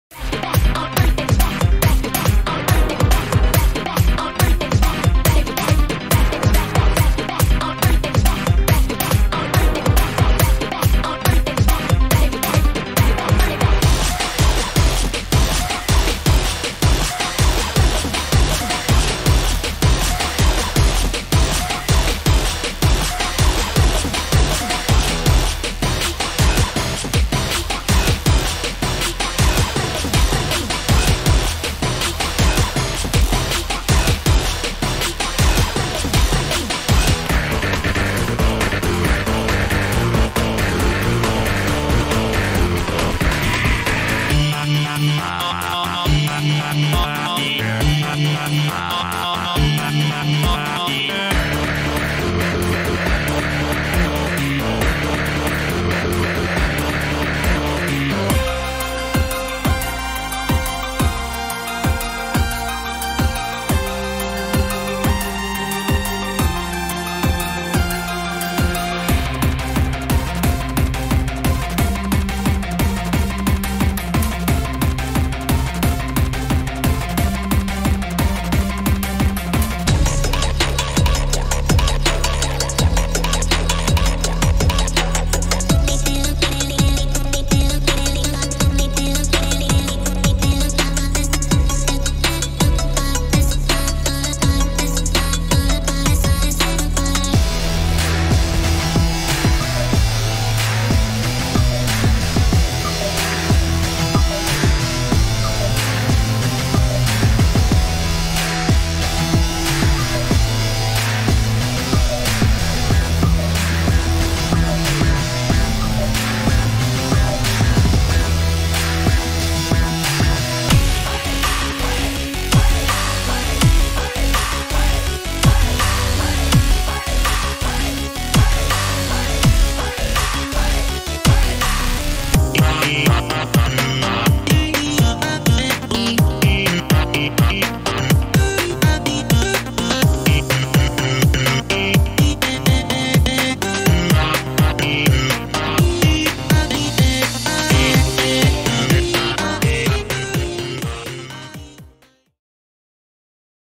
SONICSTRONGS MUSIC REEL ( OPEN COMMISIONS )
yo yo, im sonicstrong ( or Sam)
I've decided to open song commissions for a short time, as trying to find a job where i live is like trying to find the damn one piece. this video is to show some examples of my work before/ if you decide to hire me!
I'm an aspiring artist, and over the last 3 years I've been learning to make music in FL studio. i can do mainly electronic, metal and trap music best as it stands.
once your song is done and sent you are free to use it for whatever you like as long as proper credit is given
1-2 min instrumental: $25
3 min instrumental song: $32
any extra min: $12
remix of a pre existing song: $60
+ fnf vocals : $10
payment must be upfront before i begin the prosses on your song.
you may request certain styles or samples to be used if you choose to do so. no extra cost btw